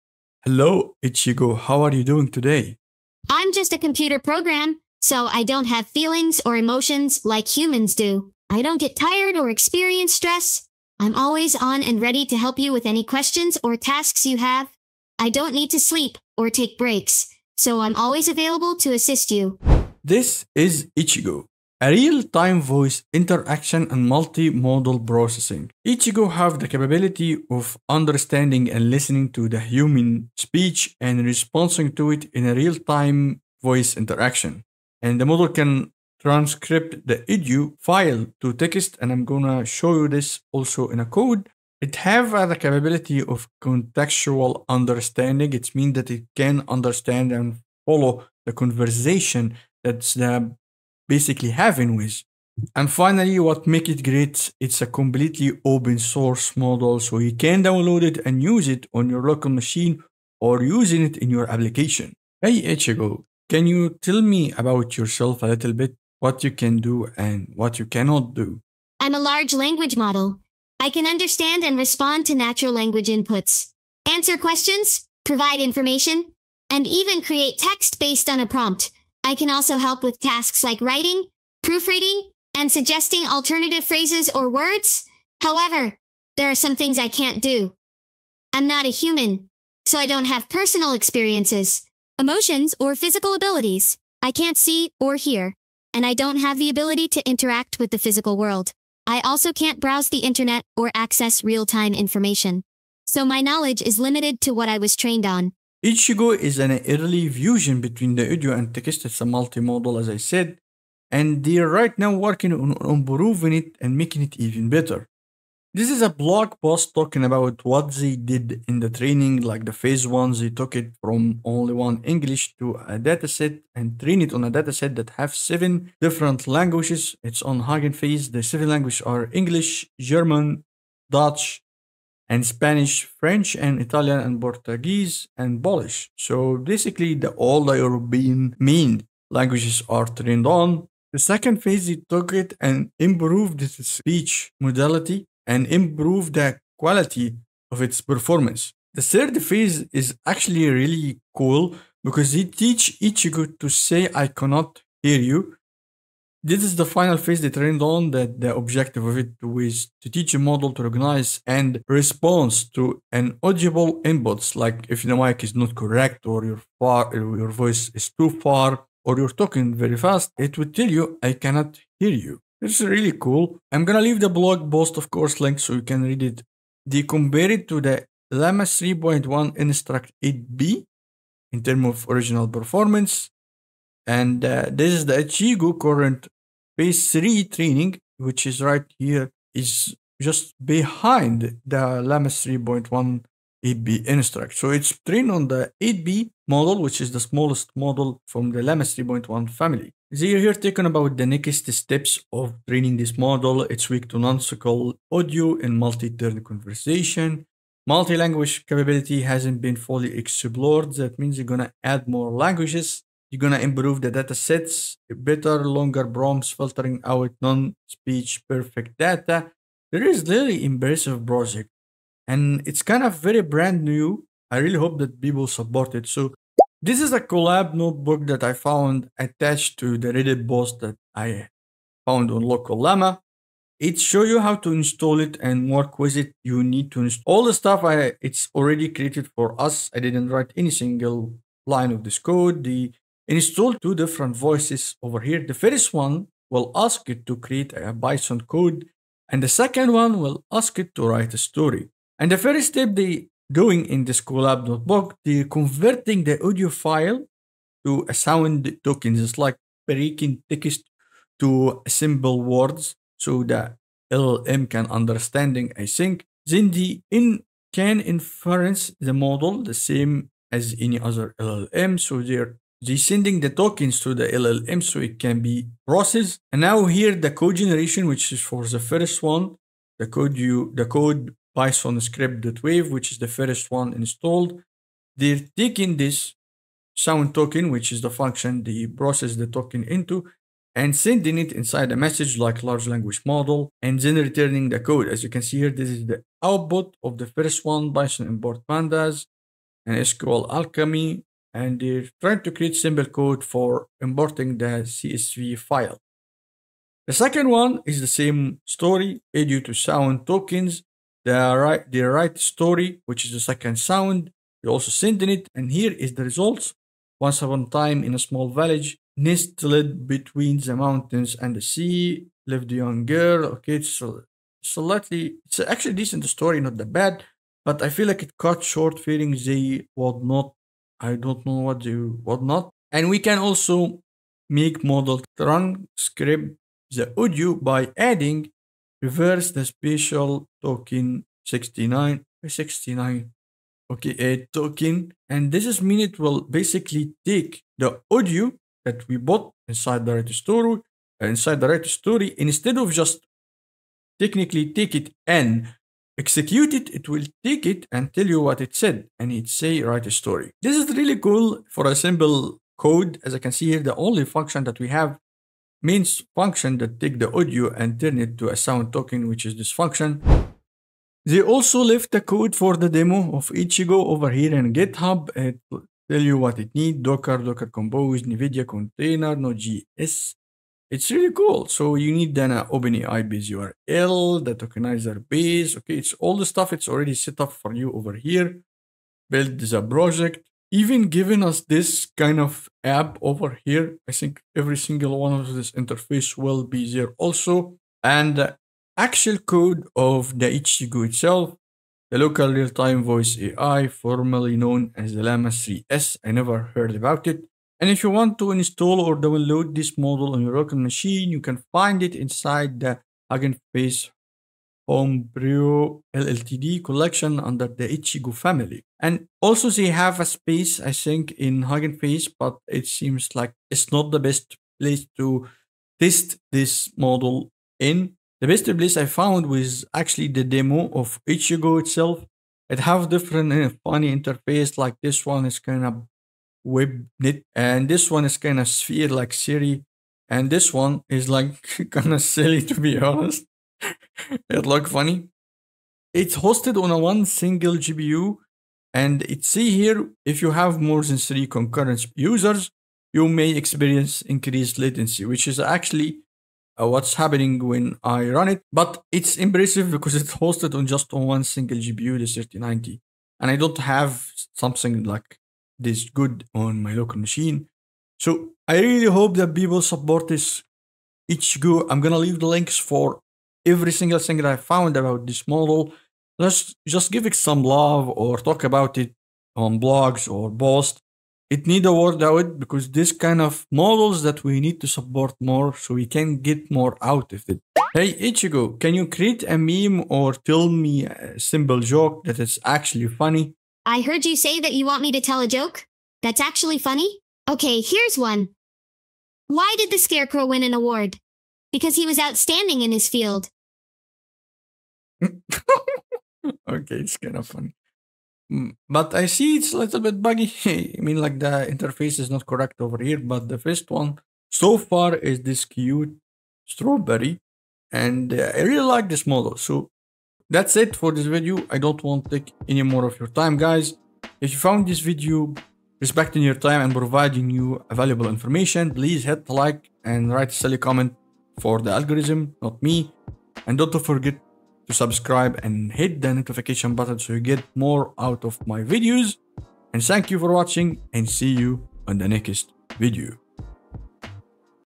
Ichigo-Llama3.1: Local Real-Time Voice AI
Explore the power of Ichigo-Llama3.1, a cutting-edge local real-time voice AI. This video covers everything you need to know, from its capabilities and training process to running it on Google Colab. Learn how to access the repository, download the software, and see a live demo in action.

00:00 - Ichigo-Llama3.1
00:22 - What is Ichigo-Llama
01:01 - Ichigo-Llama3.1 What it can do
01:50 - Ichigo-Llama Training
03:47 - Ichigo-Llama Running on Google Colab
06:10 - Ichigo-Llama Repo and Code
06:46 - Download Ichigo-Llama
07:13 - Demo  Ichigo-Llama